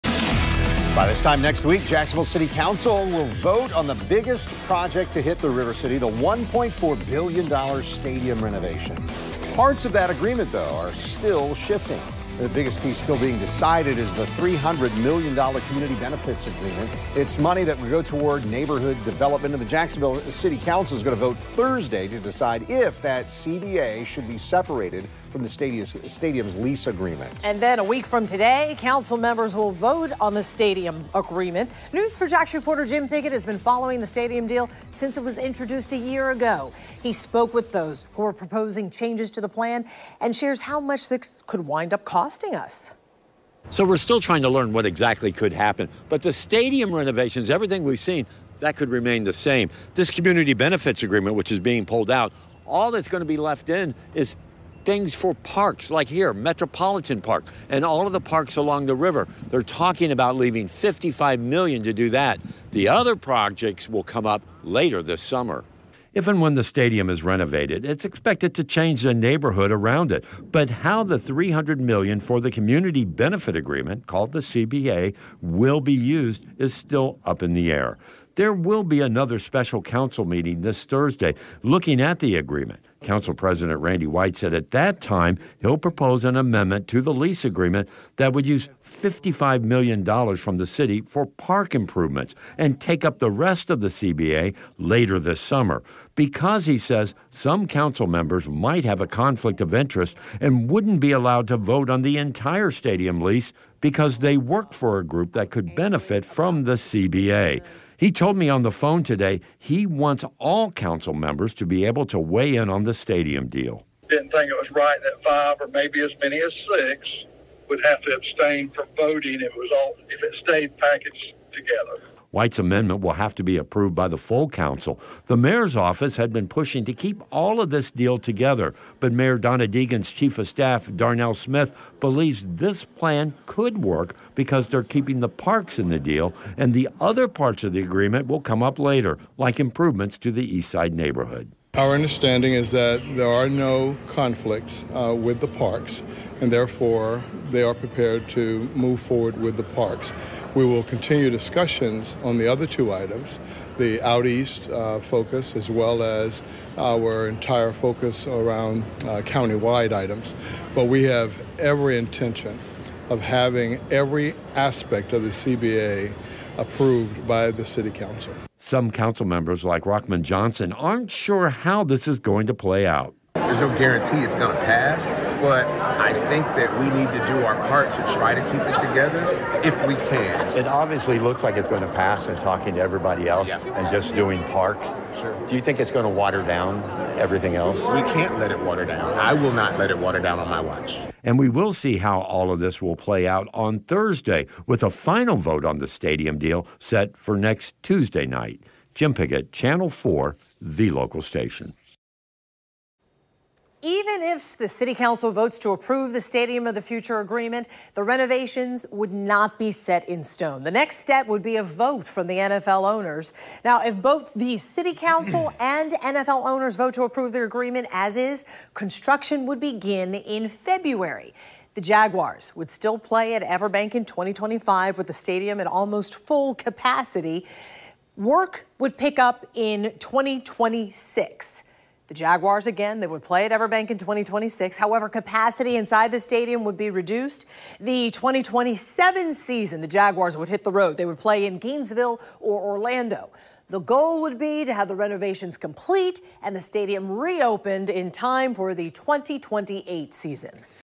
City council expected to move $94M of $150M out of stadium deal in compromise
The most controversial part of the stadium lease agreement between the Jacksonville Jaguars and the city is the $300 million Community Benefit Agreement (CBA) that would help developments outside the stadium.